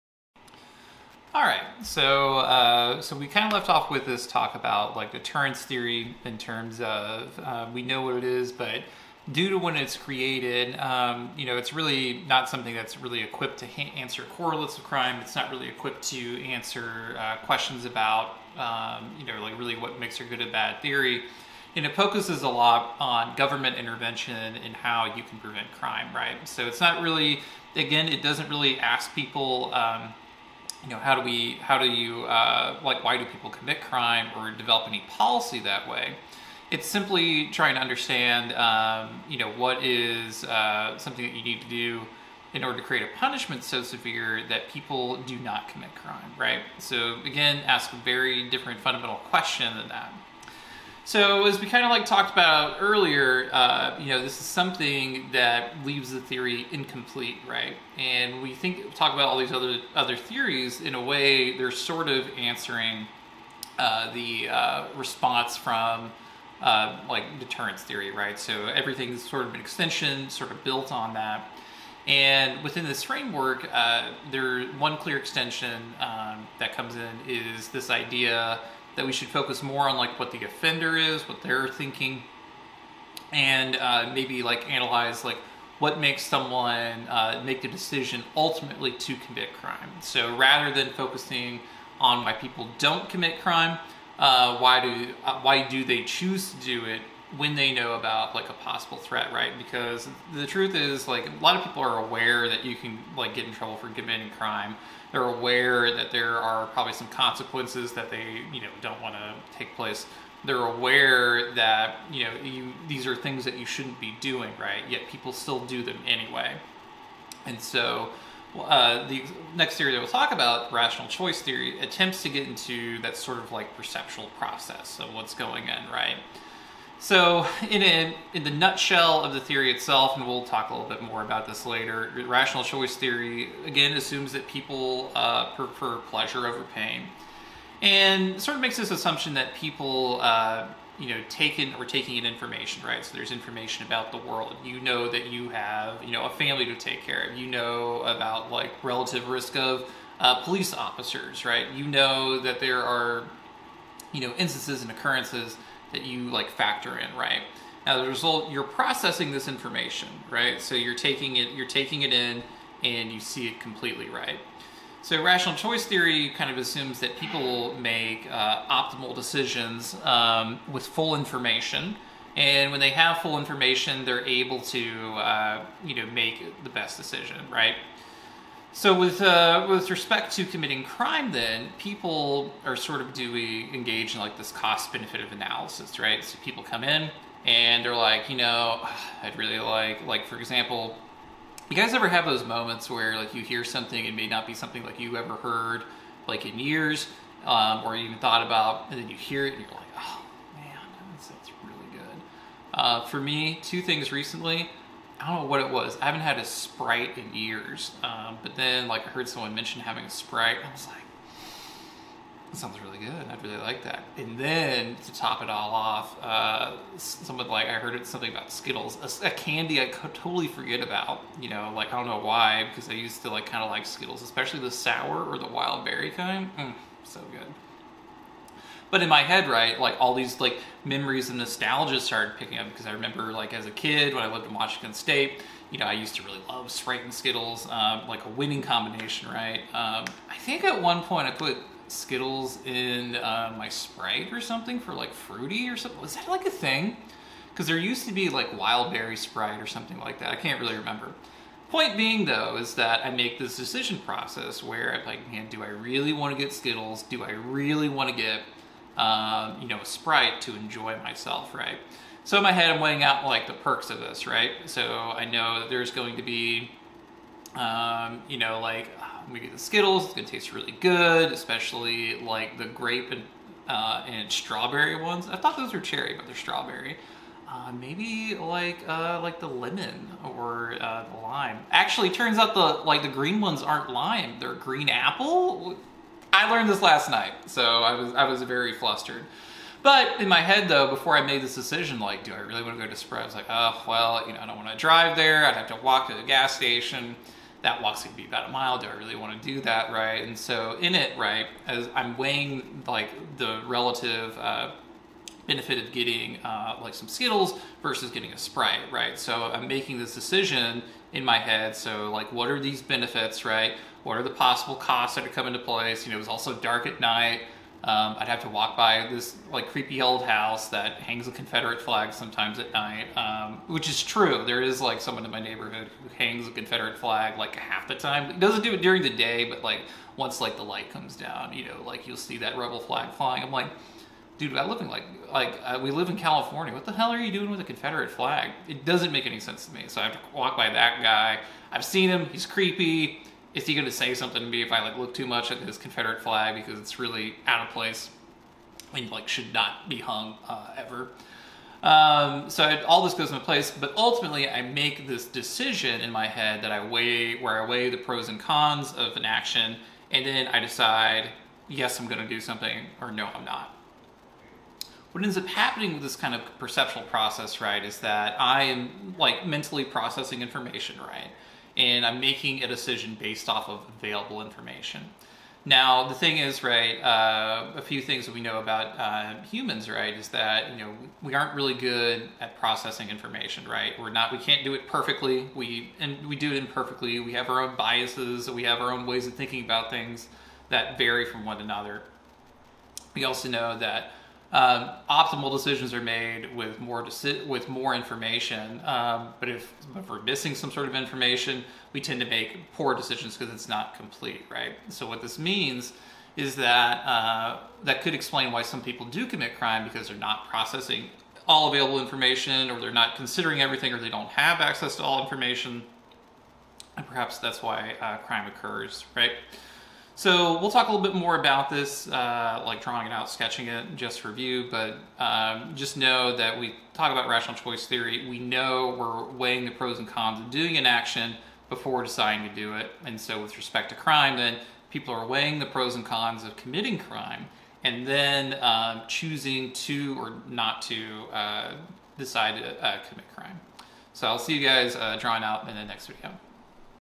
CJUS 3320 - Rational Choice, Deterrence Part 6
Introduces the core idea of rational choice theory